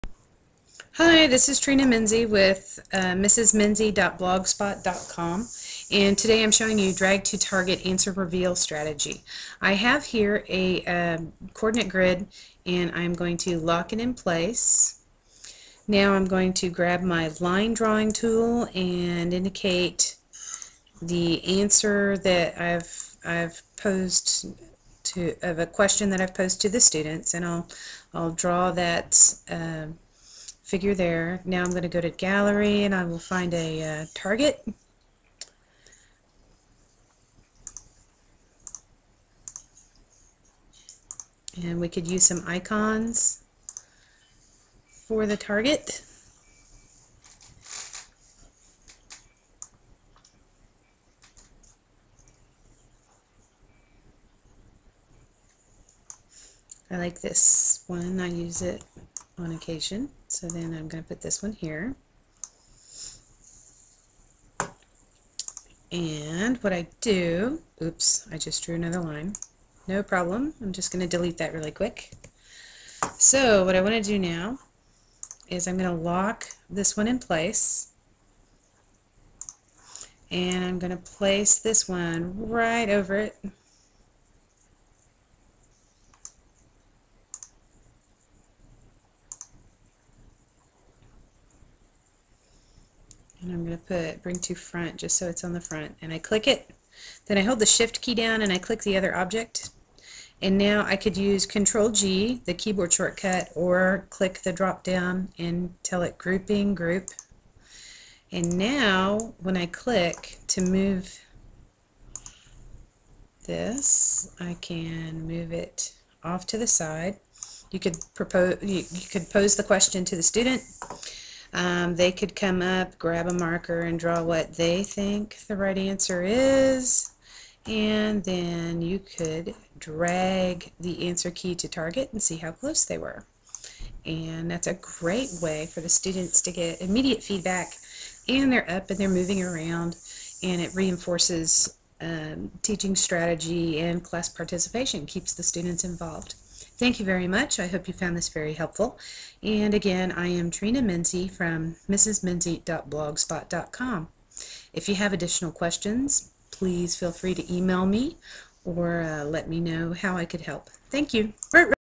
Drag to Target
SmartNotebook Answer Reveal Strategies tutorial video, using the grouping of separate items overlayed over questions or graphics, then dragging the answer key into the target position.